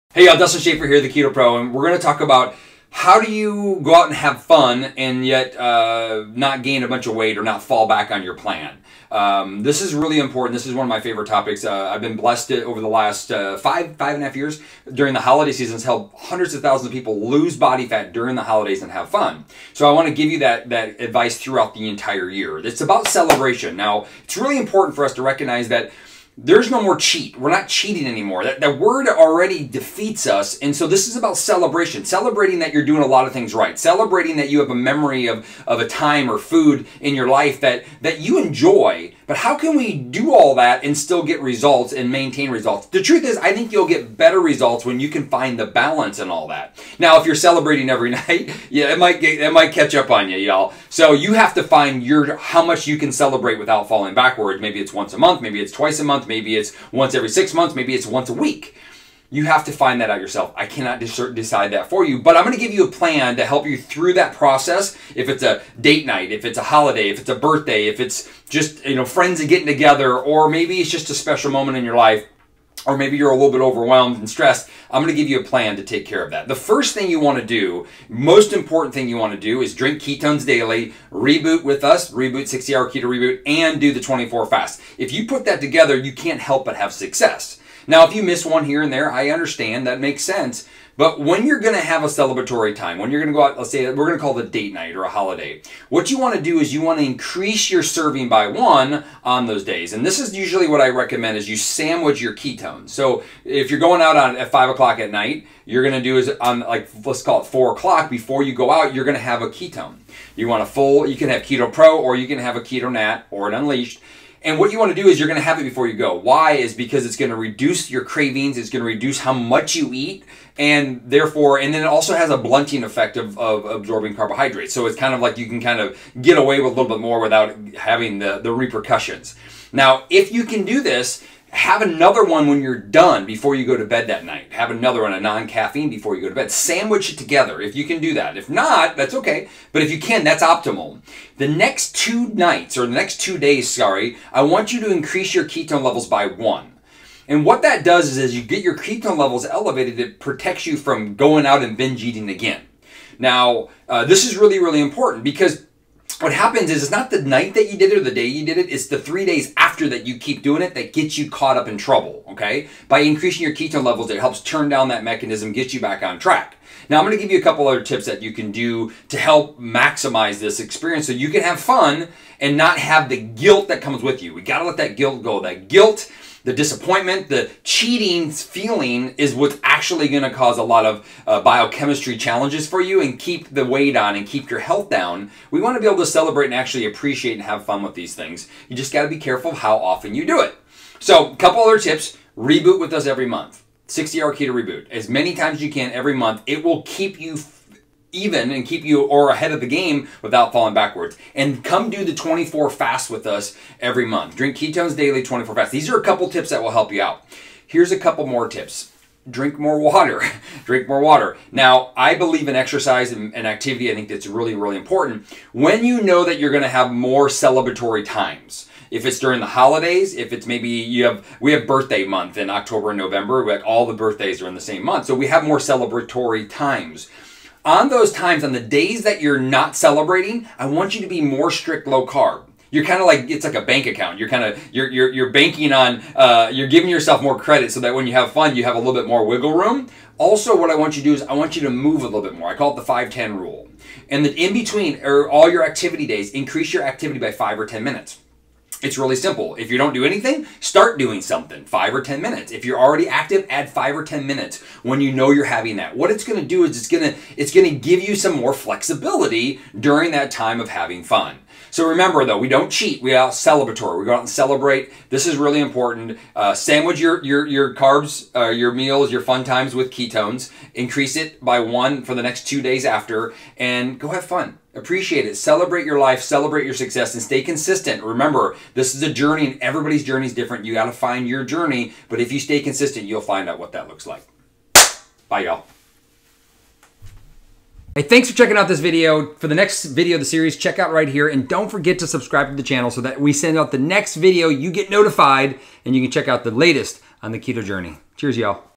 How to do keto on vacation and holidays | Have Fun on Keto | Dustin Schaffer The Keto Pro
Wondering what happens when you fall off the keto wagon? It is entirely possible to include cheat meals on your keto diet and still lose weight. But how drastically and how often you have a keto diet cheat is certainly going to affect your progress in the long run, and you might end up undoing a lot of hard work if you aren't careful.

Here is everything you need to know about keto cheat days including what happens when you cheat and how to enjoy "forbidden" cheat meals the right way.